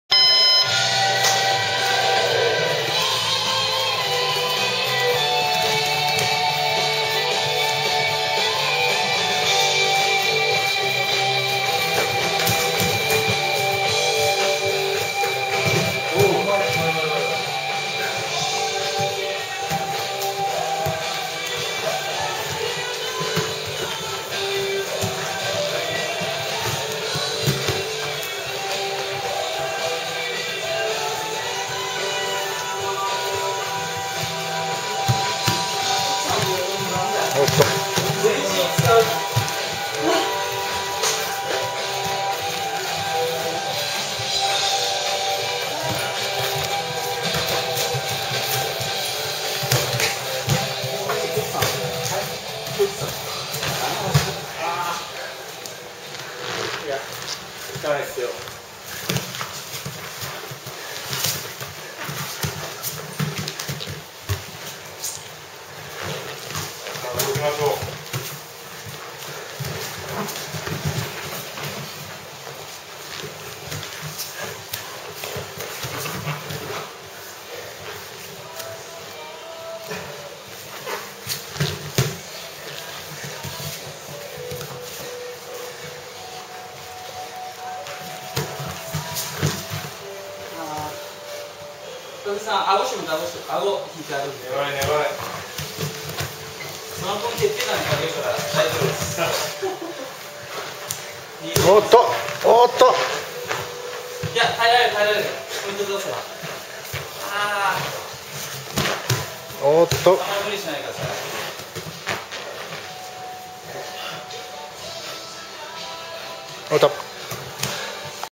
Sasaki vs Sano (2010-08-28)
Sparring in NO GI BJJ (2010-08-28) #2

Sasaki (Black/Pink) : 65kg
Sano (Black/White) : 78kg